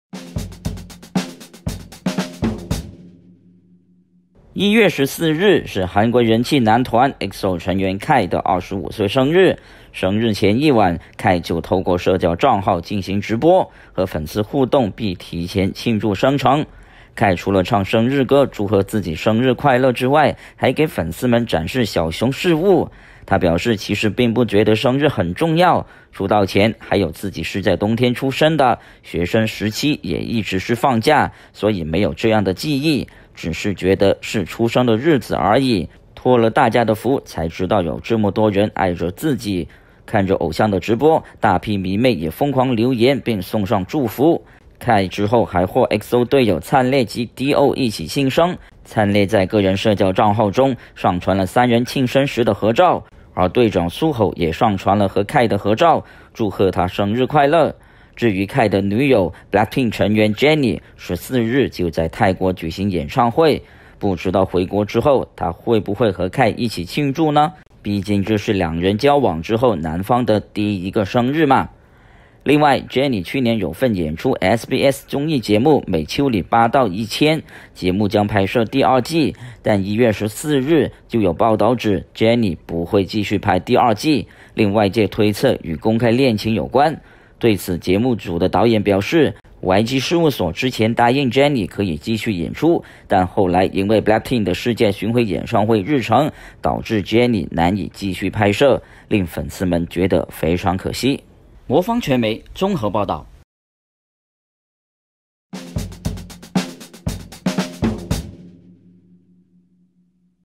2019-01-14 Kai獲EXO隊友慶祝25歲生日 女友Jennie因戀情退出節目？
1月14日
是韓國人氣男團EXO成員Kai的25歲生日
生日前一晚
Kai就透過社交帳號進行直播
和粉絲互動並提前慶祝生辰
Kai除了唱生日歌祝賀自己生日快樂外
還給粉絲們展示小熊飾物
他表示其實並不覺得生日很重要
出道前 還有自己是在冬天出生的
學生時期也一直是放假
所以沒有這樣的記憶
只是覺得是出生的日子而已
托了大家的福
才知道有這麼多人愛著自己
看著偶像的直播
大批迷妹也瘋狂留言並送上祝福
Kai之後還獲EXO隊友燦烈及D.O.一起慶生
燦烈在個人社交帳號中
上傳了三人慶生時的合照
而隊長Suho也上傳了和Kai的合照
祝賀他生日快樂
至於Kai的女友
BLACKPINK成員Jennie
14日就在泰國舉行演唱會
不知道回國後
她會不會和Kai一起慶祝呢
畢竟這兩人交往後男方的第一個生日嘛